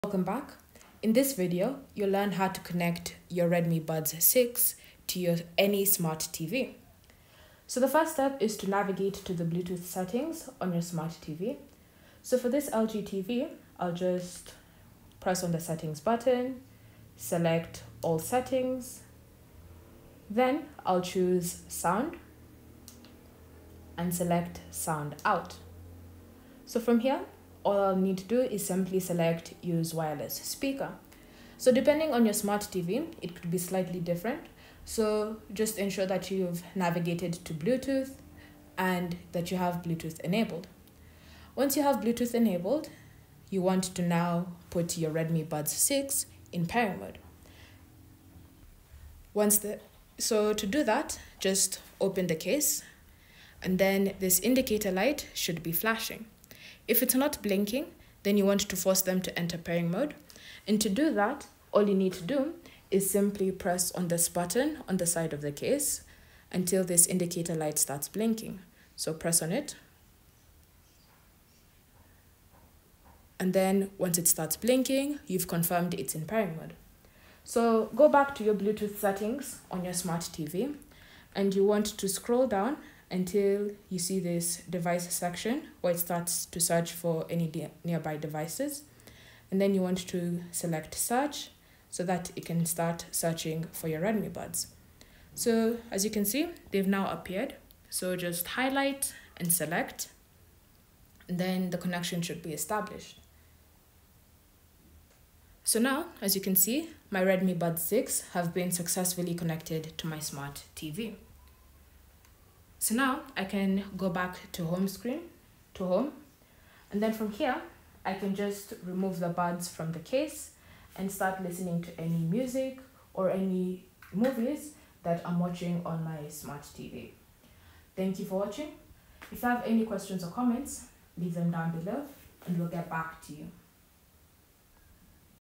How To Connect Redmi Buds 6 To ANY Smart TV
Learn How To Connect Redmi Buds 6 To ANY Smart TV. In this quick and easy tutorial, I’ll show you how to connect your Redmi Buds 6 to any smart TV for an amazing wireless audio experience!

Whether you're using Samsung, LG, Sony, or any Android TV, I’ll guide you step-by-step through the pairing process to get your Redmi Buds 6 working with your TV in no time!

Redmi Buds 6

Shokz OpenRun Pro

VIZIO 40-inch Smart TV

Main Camera (iPhone 14)
Cell Phone Tripod Adapter